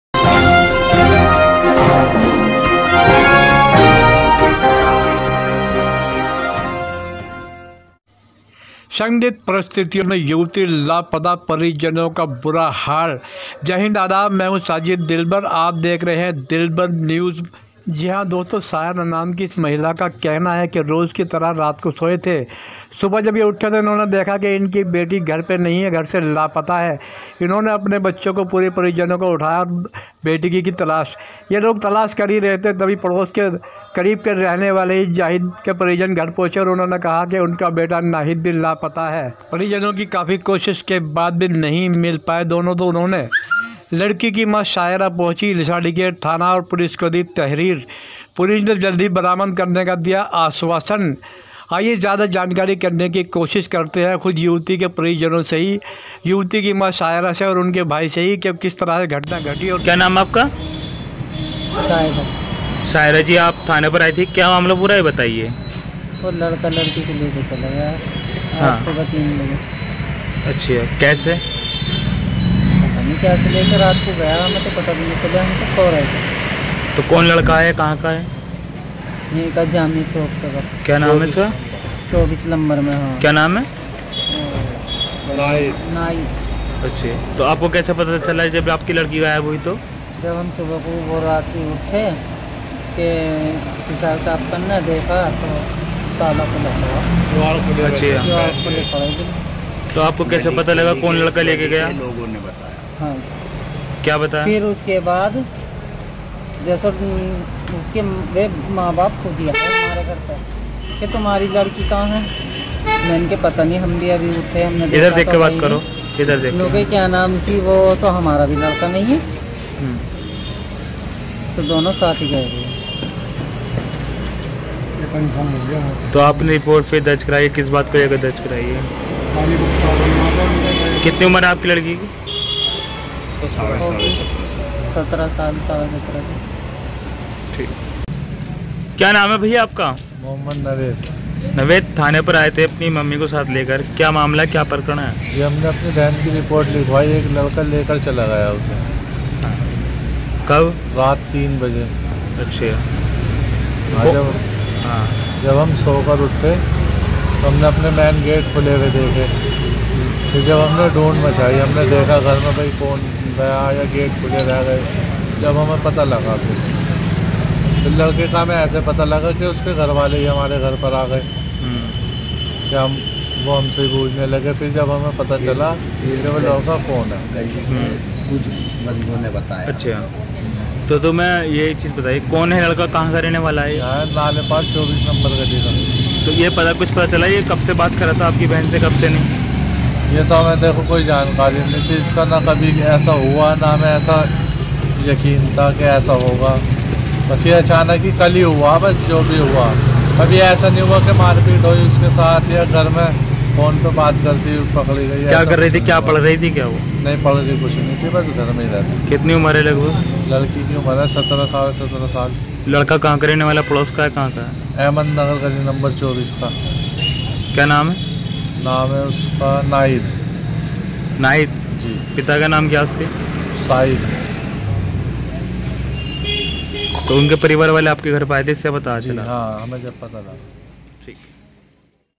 संदीतपरिस्थितियों में प्रेमी युगल लापता
युगल के लापता होने से परिजन परेशान पुलिस से लगाई गुहार जल्द बरामद करने का पुलिस का आश्वासन ज्यादा जानकारी के लिए देखें दिलबर न्यूज़